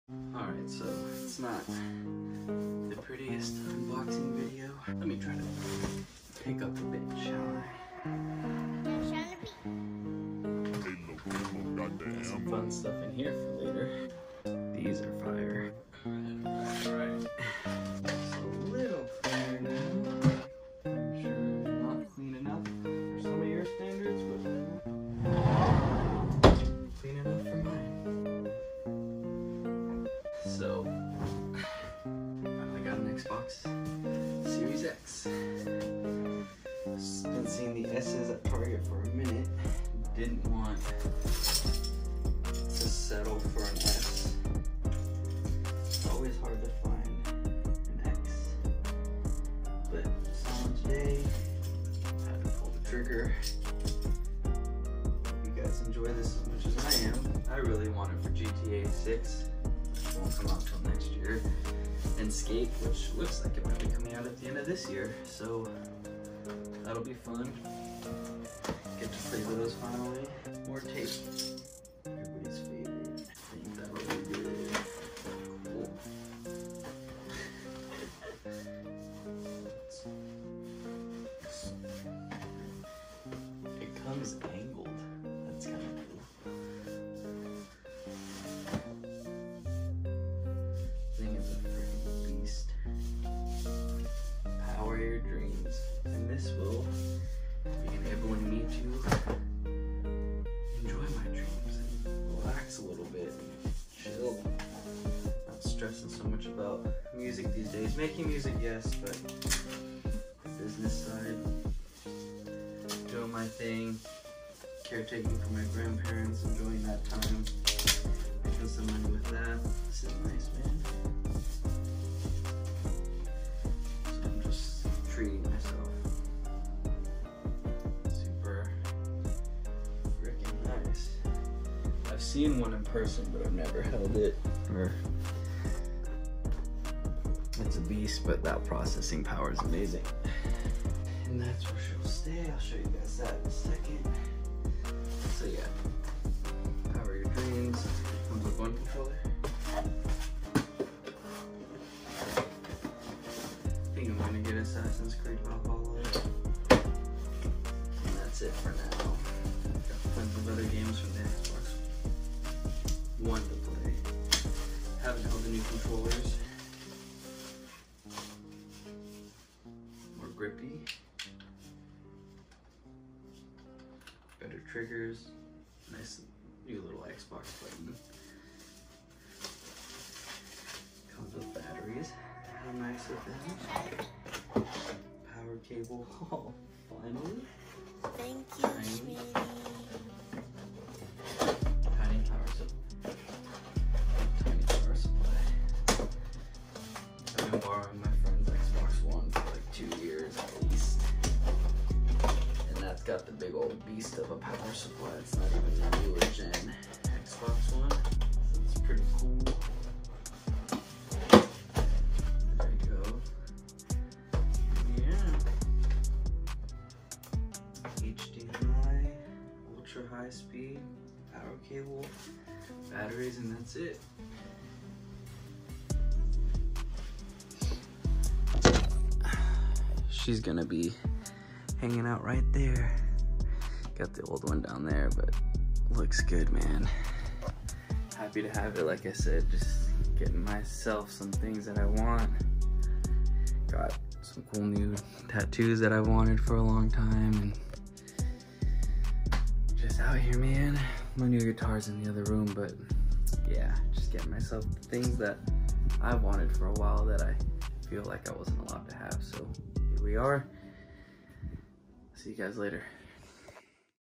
XBOX SERIES X UNBOXING
Finally got an Xbox series x from target today. Took me a while to find one but I finally did! Enjoy!